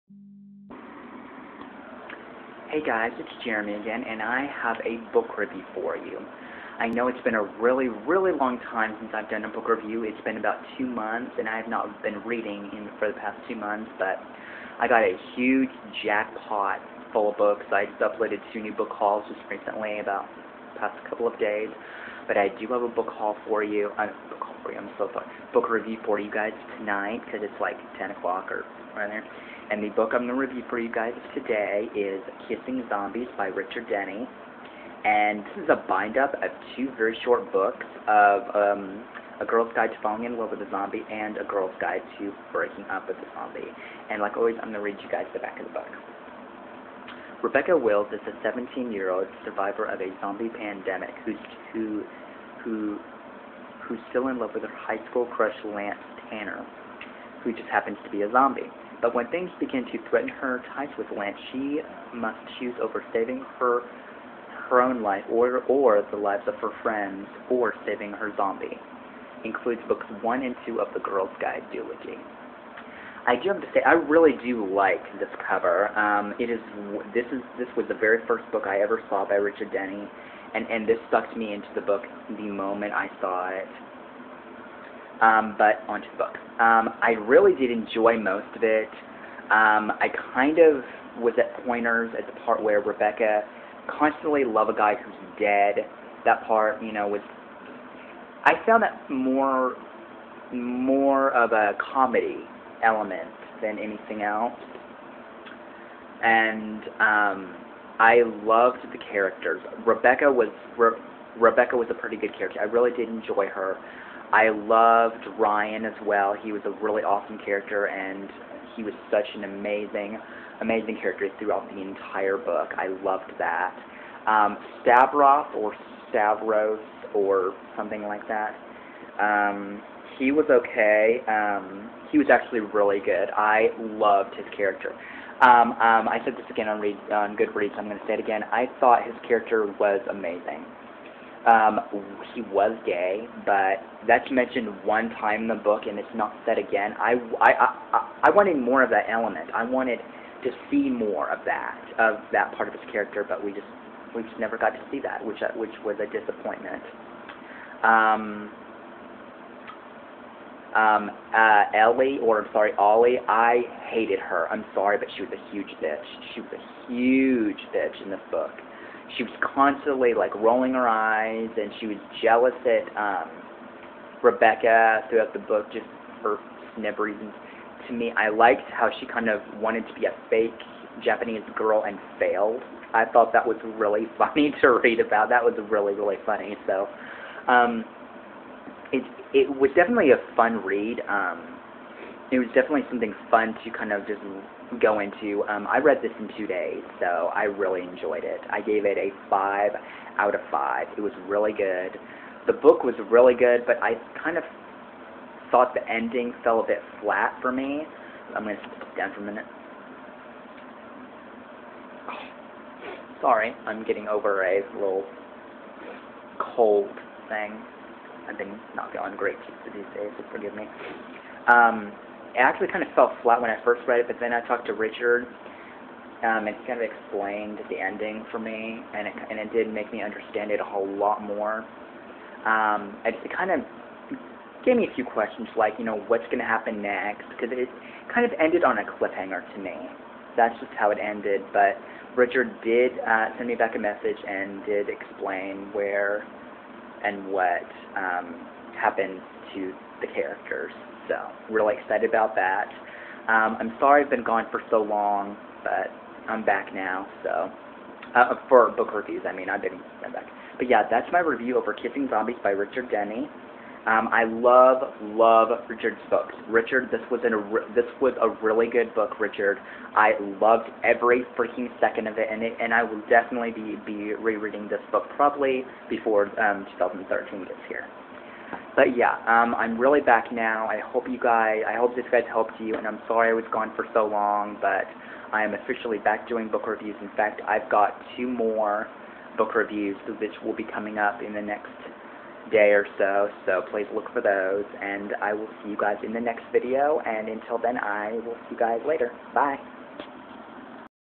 Book review : kissing zombies
Rebecca Willis is a 17 year old survivor of a zombie pandemic who's still in love with her high school crush Lance Tanner, who just happens to be be a zombie. But when things begin to threaten her ties with Lance she must choose over saving her own life and the lives of her friends or saving her zombie.